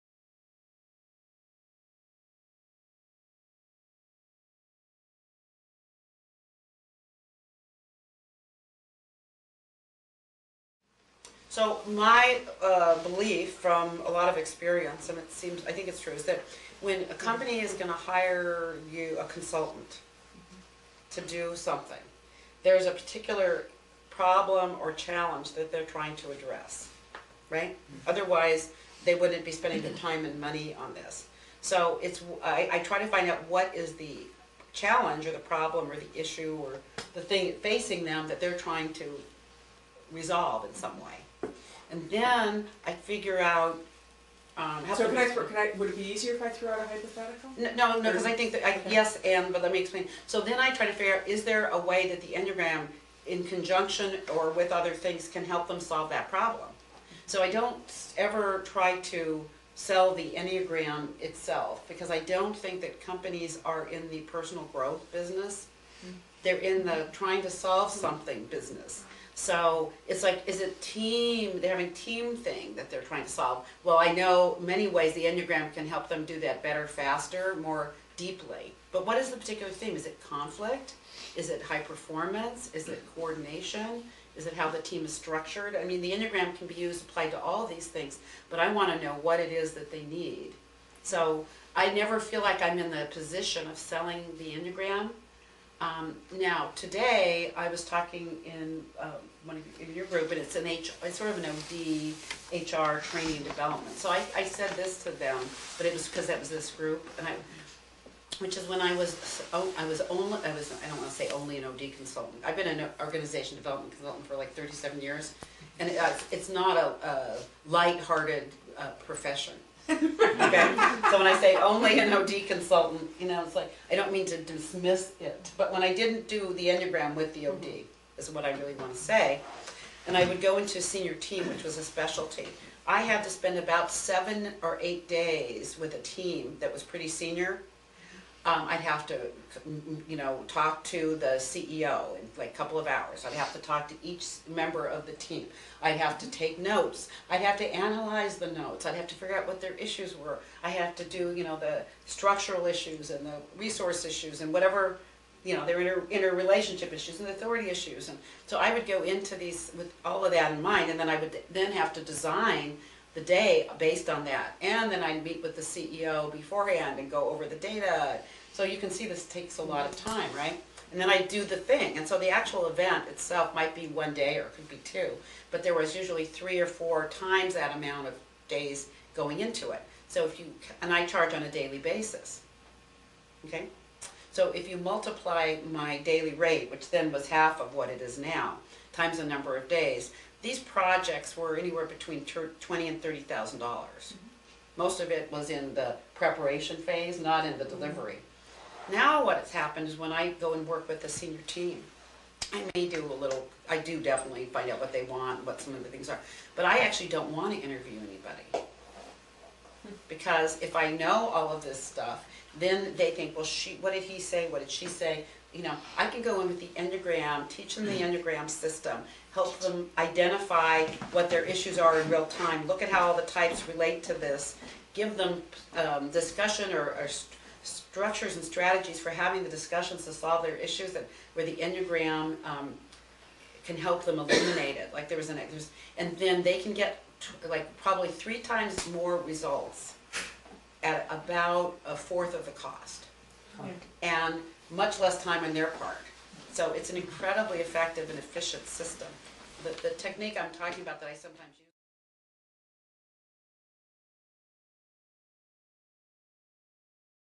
Positioning the Enneagram with Prospective Clients - Full Clip
Ginger Lapid-Bogda, Ph.D. answers the question: "How do you position the Enneagram with prospective business clients?"